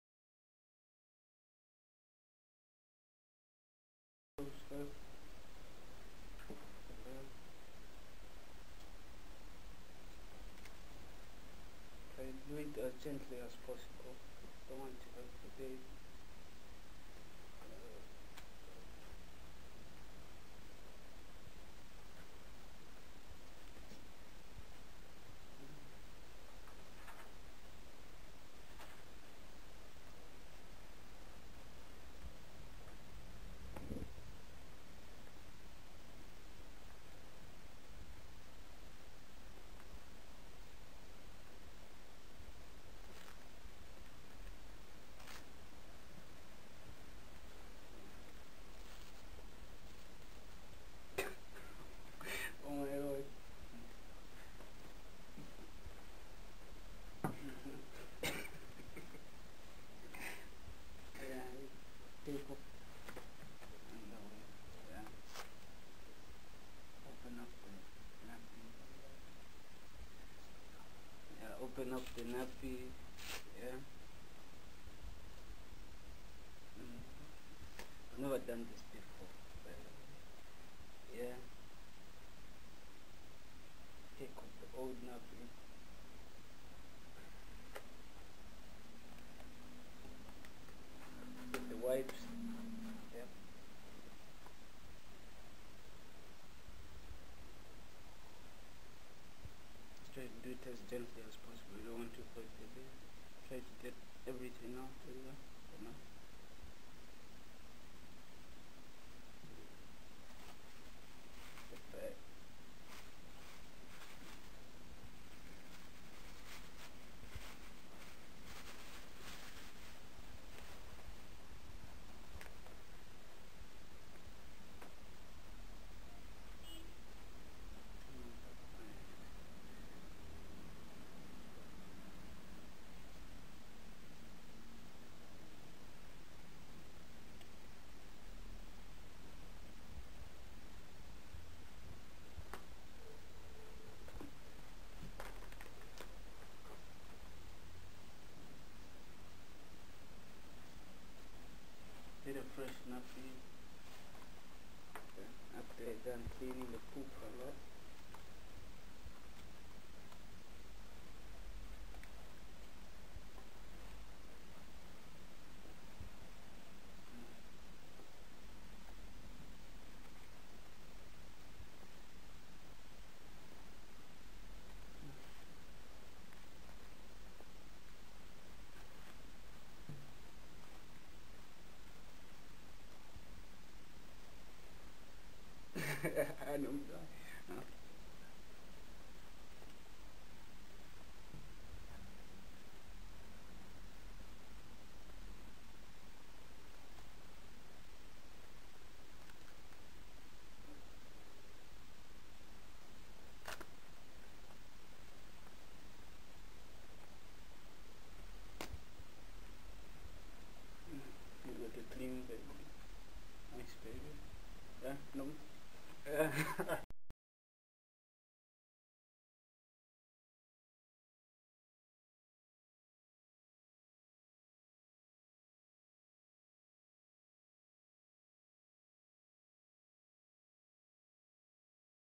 My First Nappy Change.
Sean (a young dad to be from Woolwich, London) tries changing a nappy for the first time.
Sean is supported by Working With Men & was filmed by other young dads in a session  powered by YoungDads.TV.